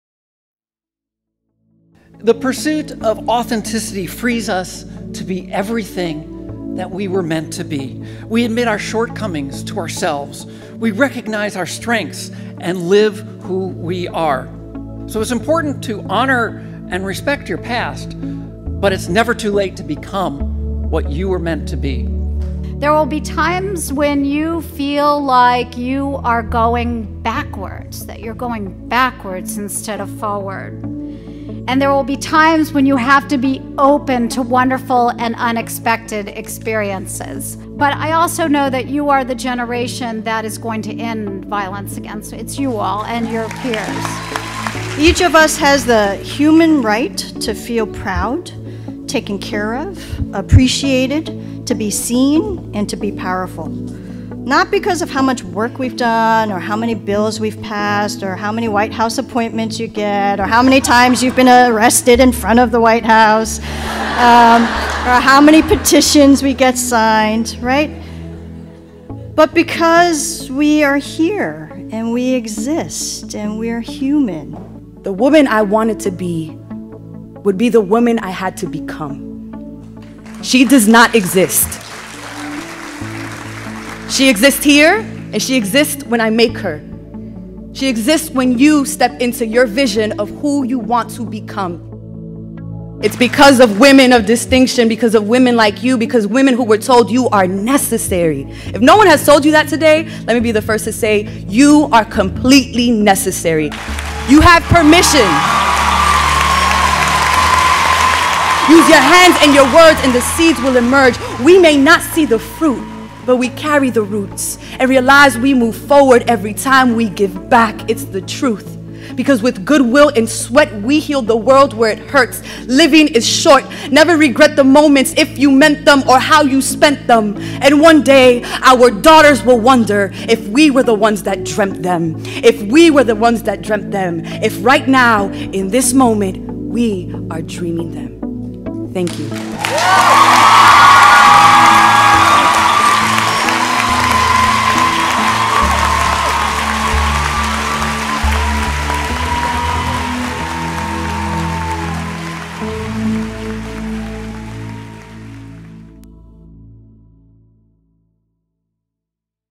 Leadership Redefined: Life Lessons from 2015 Women of Distinction
“The woman I wanted to be, would be the woman I had to become; she exists when you step into your vision of who you want to become. “ Poet Elizabeth Acevedo

Learn these life lessons and more insight from the Women of Distinction honored at the 2015 National Conference for College Women Student Leaders (NCCWSL).  Every year, five to six incredible women inspire tomorrow’s women leaders – women like you, eager to find your voice and share it with the world.

NCCWSL, hosted by AAUW at the University of Maryland, College Park, gathers college women from across the United States to network, sharpen their leadership skills, and build their resumes.  This video highlights just a fraction of the powerful and empowering messaging you’ll find at NCCWSL.    Be there in 2016.  (It’s even better than the video!)